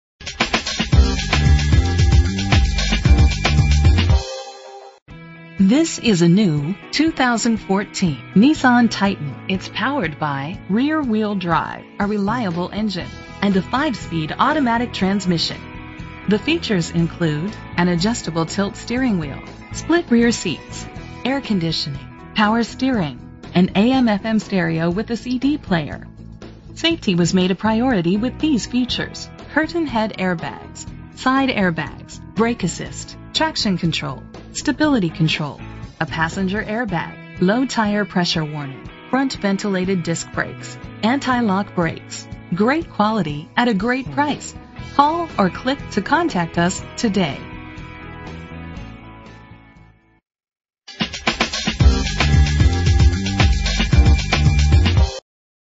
2014 Nissan Titan - Mesa AZ
Year: 2014
Make: Nissan
Model: Titan
Trim: S
Engine: 5.6 liter V-8
Transmission: 5-Speed Automatic
Color: White
Mileage: 1
Address:
6354 E Test Drive